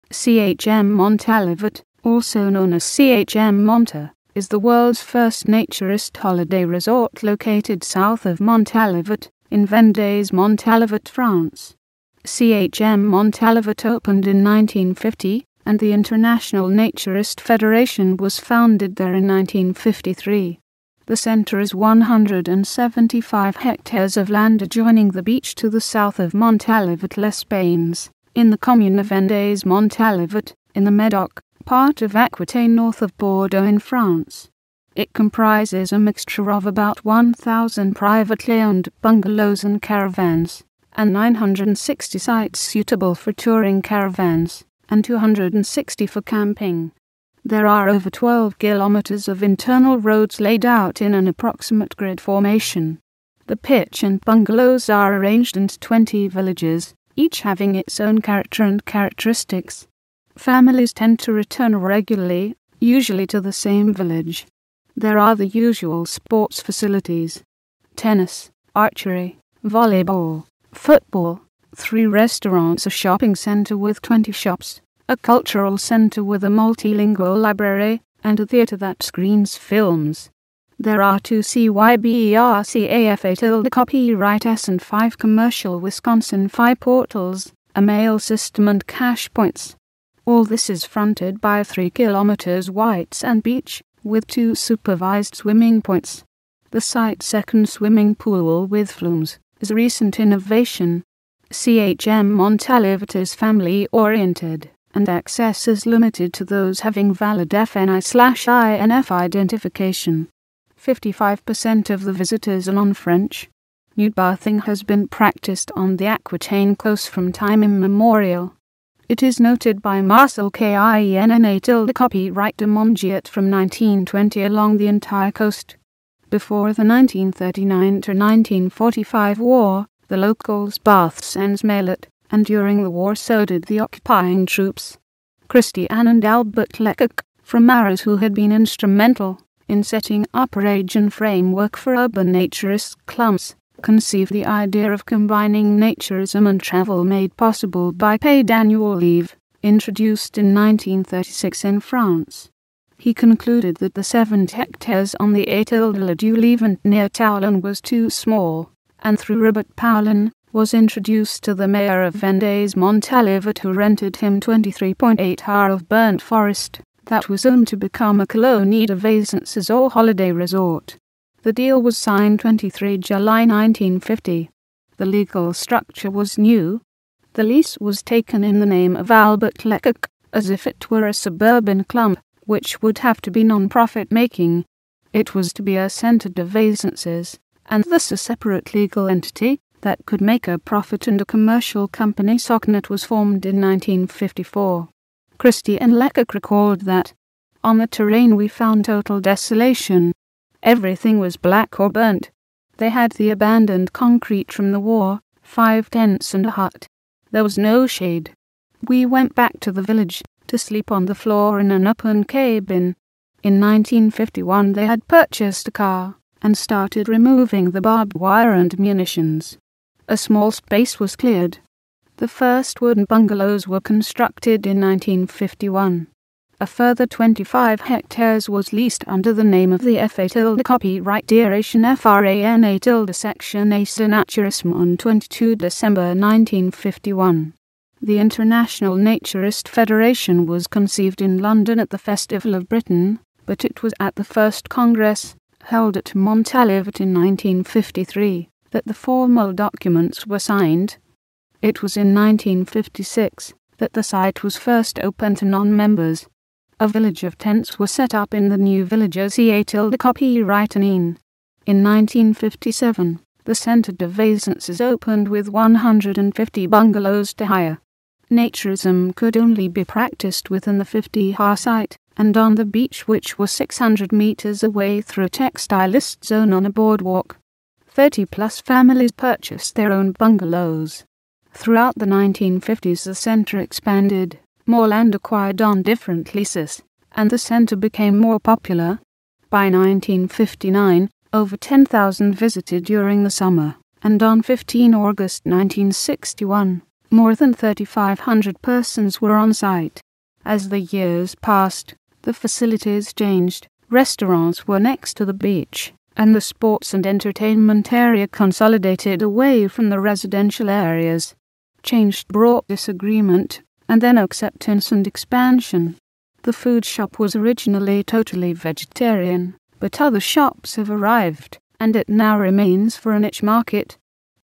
CHM Montalivet
CHM Montalivet, also known as CHM Monta, is the world's first naturist holiday resort located south of Montalivet, in Vendays-Montalivet, France. CHM Montalivet opened in 1950, and the International Naturist Federation was founded there in 1953.

The centre is 175 hectares of land adjoining the beach to the south of Montalivet-les-Bains, in the commune of Vendays-Montalivet, in the Medoc, part of Aquitaine north of Bordeaux in France. It comprises a mixture of about 1000 privately owned bungalows and caravans, and 960 sites suitable for touring caravans and 260 for camping. There are over 12 km of internal roads laid out in an approximate grid formation. The pitch and bungalows are arranged into 20 villages, each having its own character and characteristics. Families tend to return regularly, usually to the same village.

There are the usual sports facilities: tennis, archery, volleyball, football, three restaurants a shopping centre with 20 shops, a cultural centre with a multilingual library and a theatre that screens films. There are two cybercafés and five commercial WI-FI portals, a mail system and cash points. All this is fronted by a 3 km white sand beach, with two supervised swimming points. The site second swimming pool with flumes is a recent innovation.

CHM Montalivet is family-oriented, and access is limited to those having valid FNI/INF identification. 55% of the visitors are non-French.

Nude bathing has been practised on the Aquitaine coast from time immemorial. It is noted by Marcel Kienné de Mongeot from 1920 along the entire coast. Before the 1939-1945 war, the locals bathed 'sans maillot', and during the war so did the occupying troops. Christiane and Albert Lecocq, from Arras who had been instrumental in setting up a region framework for urban naturist clubs, conceived the idea of combining naturism and travel made possible by paid annual leave, introduced in 1936 in France. He concluded that the 70 hectares on the Île du Levant near Toulon was too small, and through Robert Poulain was introduced to the mayor of Vendays-Montalivet who rented him 23.8 ha of burnt forest that was zoned to become a 'Colonie de Vacances' or holiday resort. The deal was signed 23 July 1950. The legal structure was new. The lease was taken in the name of Albert Lecocq as if it were a suburban club, which would have to be non profit making. It was to be a 'Centre de Vacances' and thus a separate legal entity that could make a profit and a commercial company SOCNAT was formed in 1954.

Christiane Lecocq recalled that: 'On the terrain we found total desolation. Everything was black or burnt.' They had the abandoned concrete from the war, five tents and a hut. There was no shade. We went back to the village to sleep on the floor in an open cabin'. In 1951 they had purchased a car, and started removing the barbed wire and munitions. A small space was cleared. The first wooden bungalows were constructed in 1951. A further 25 hectares was leased under the name of the Féderation Français de Naturisme on 22 December 1951.

The International Naturist Federation was conceived in London at the Festival of Britain but it was at the first congress, held at Montalivet in 1953, that the formal documents were signed.

It was in 1956 that the site was first opened to non-members. A village of tents was set up in the new village 'Océanien'. In 1957, the 'Centre de Vacances' opened with 150 bungalows to hire. Naturism could only be practised within the 50 ha site and on the beach which was 600 metres away through a textilist zone on a boardwalk. Thirty-plus families purchased their own bungalows.

licensed with CC Attribution-ShareAlike 3.0. This video is licensed with CC Attribution-Share-Alike 3.0 In order to adapt this content it is required to comply with the license terms.